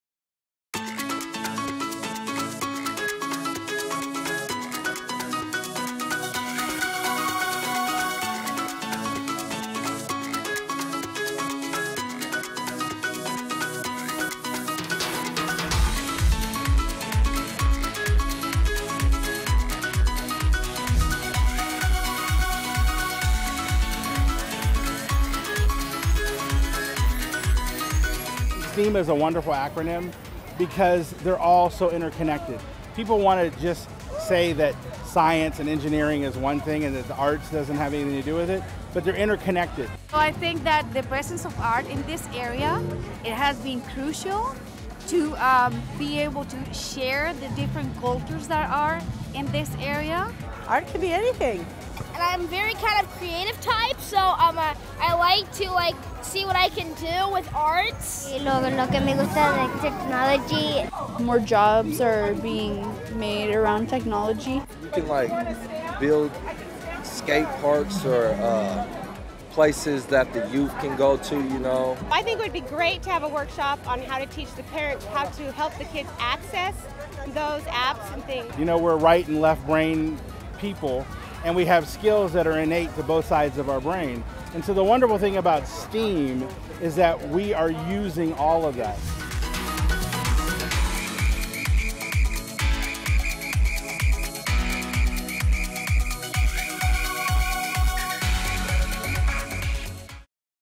Video Voice Booth Rock the Block COMPILATION 2
MetroEast took The Video Voice Booth out into the community to hear what people are thinking about issues in our neighborhoods!  People had a lot to say at Rock the Block on June 3 and 4, take a listen! And don't forget, MetroEast has low cost classes for everyone, come make media with us!  www. MetroEast.org